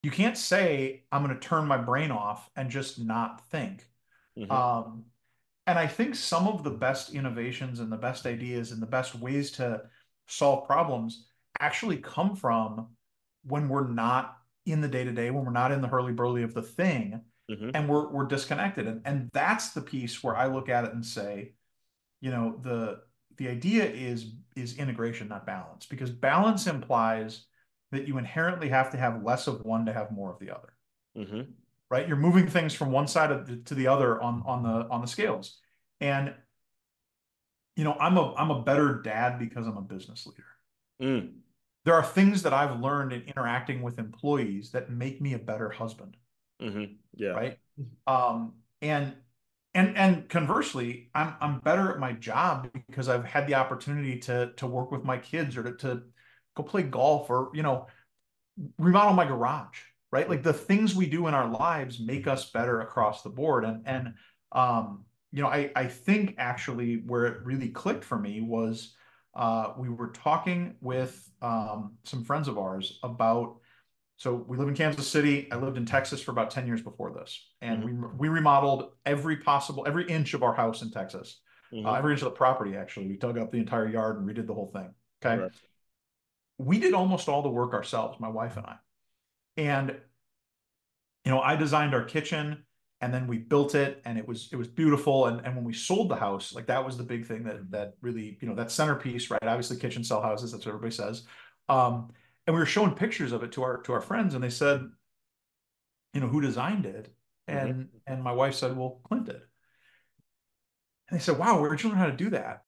Unlocking Creativity: How Disconnecting Can Lead to Innovative Ideas
Discover the secret to unlocking your creativity by disconnecting from the daily grind. Learn how integrating different aspects of your life can fuel innovation and problem-solving. Drawing inspiration from personal experiences, this video provides valuable insights and practical tips.